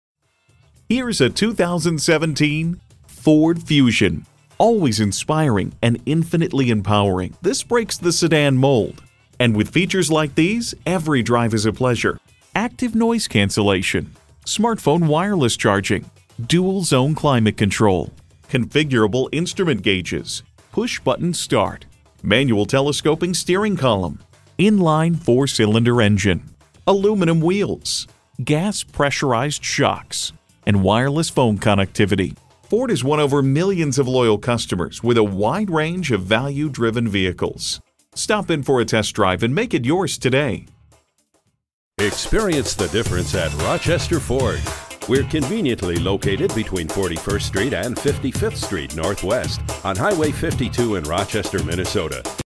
Used 2017 Ford Fusion Hybrid Rochester MN Winona, MN P9969 - SOLD
Year: 2017
Make: Ford
Model: Fusion Hybrid
Trim: SE
Engine: I4 Hybrid
Transmission: eCVT
Color: Blue
Interior: Ebony
Mileage: 50658
Stock #: P9969
VIN: 3FA6P0LU6HR285972
Recent Arrival! Fusion Hybrid SE Lightning Blue Moonroof CARFAX One-Owner. Clean CARFAX. Sunroof/Moonroof, Equipment Group 600A, Power Moonroof, Universal Garage Door Opener. Priced below KBB Fair Purchase Price! We look forward to earning your business.

Address:
4900 Highway 52 North